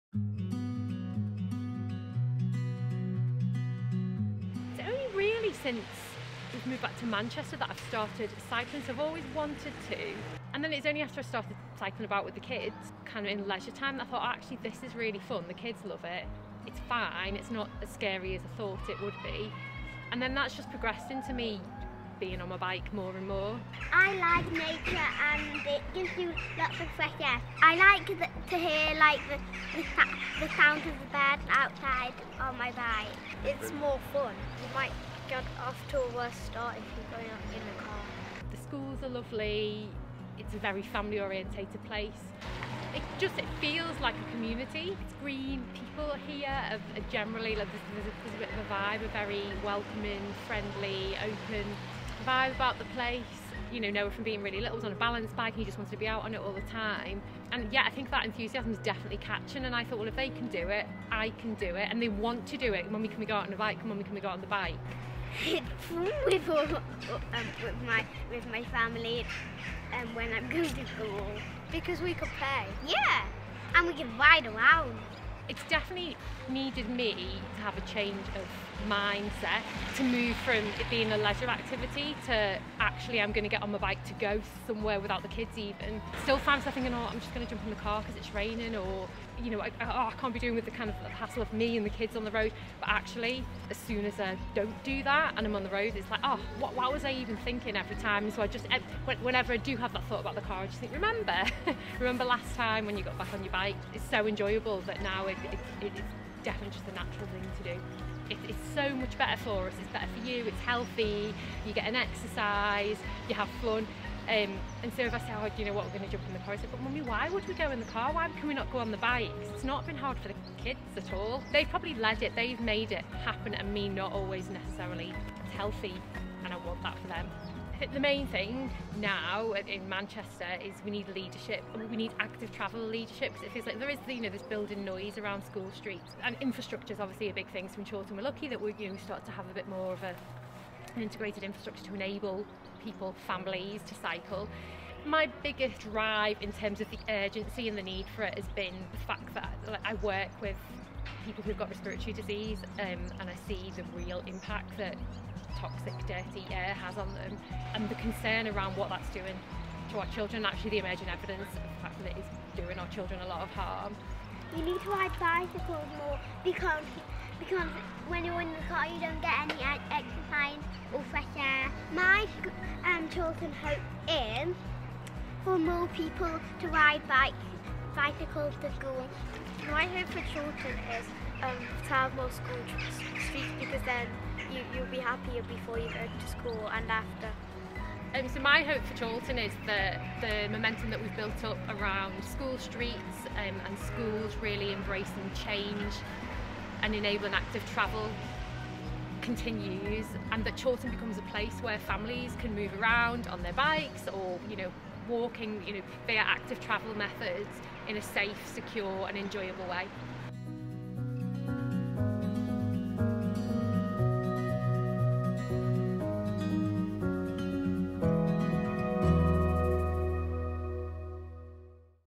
Safe space to cycle - one Manchester family share their hopes
To be able to get to school, local shops or park safely  by bike - that's what this family in Manchester are hoping to see more of.